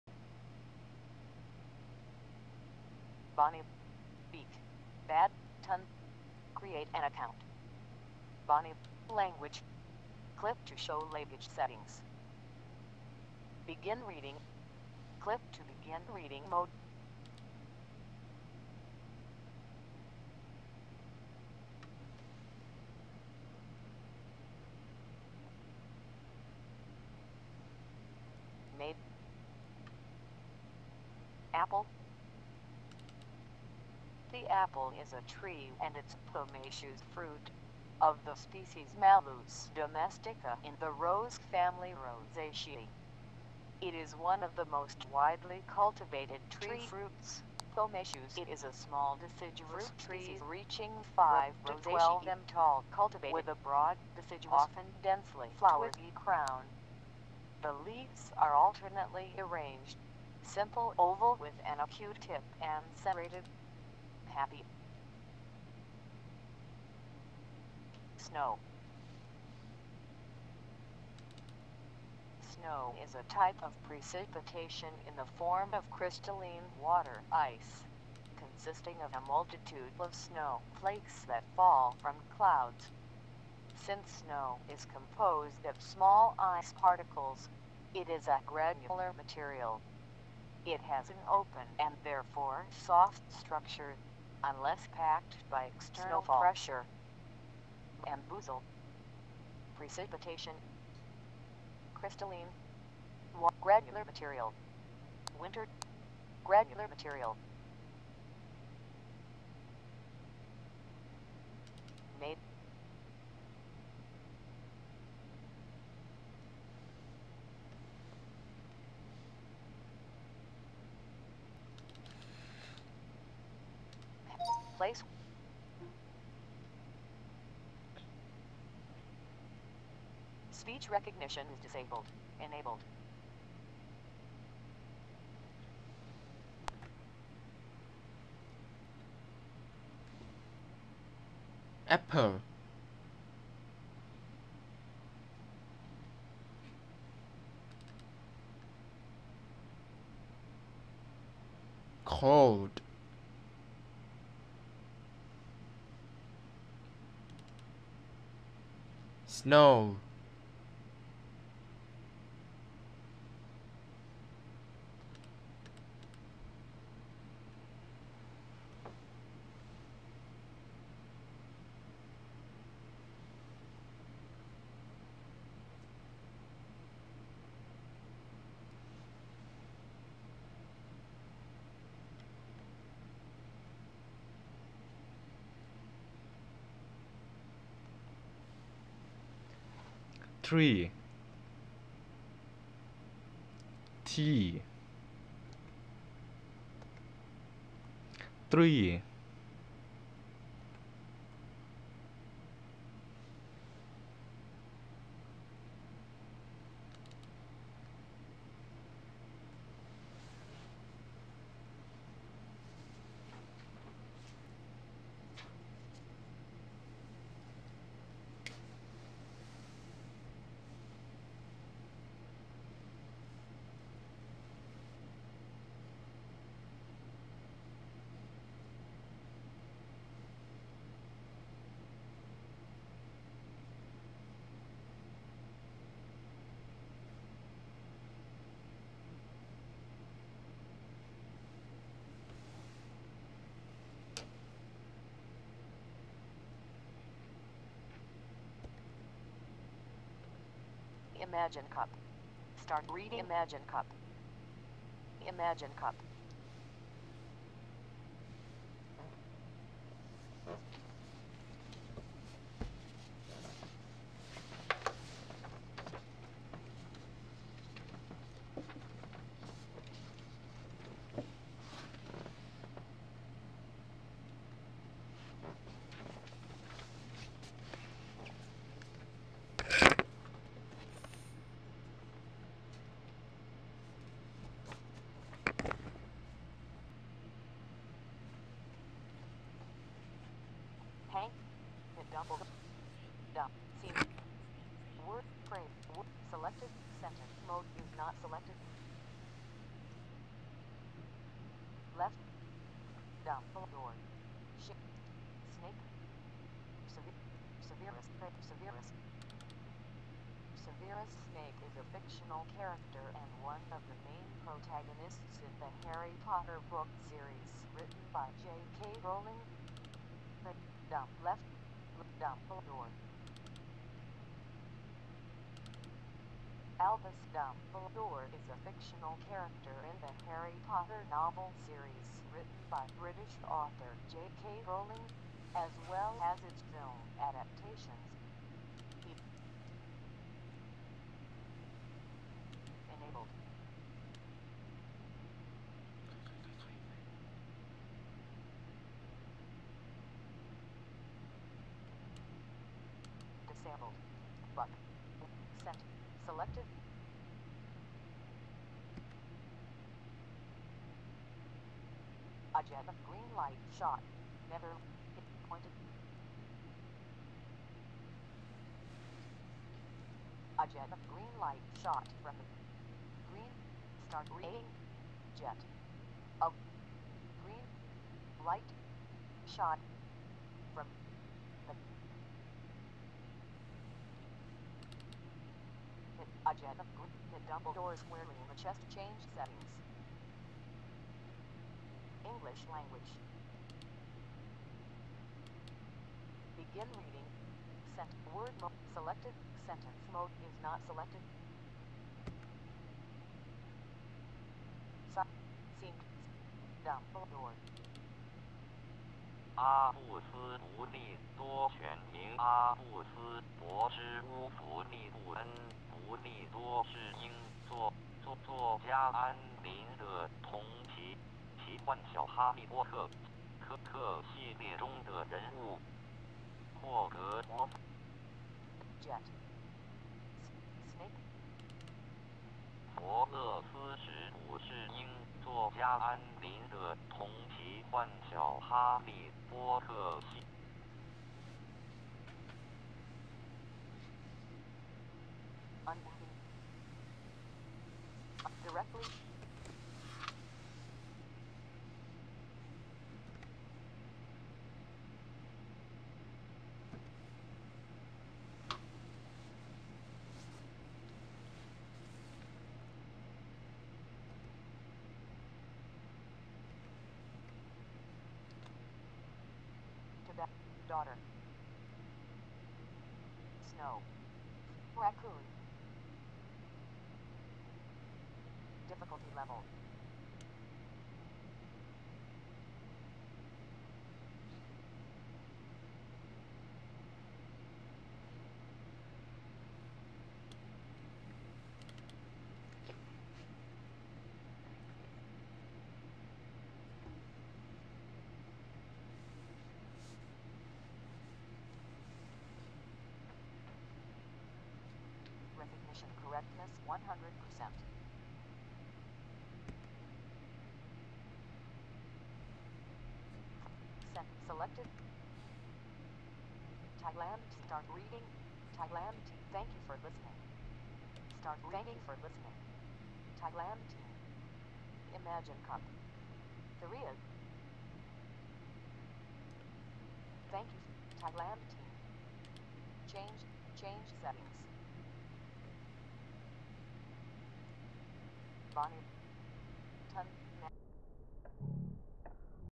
Imagine Cup Winner 2007 - Live Book
Utilize .NET Framework, WPF,Speech API and OCR. Developed by Thailand team, winner of Microsoft Imagine Cup 2007 worldwide final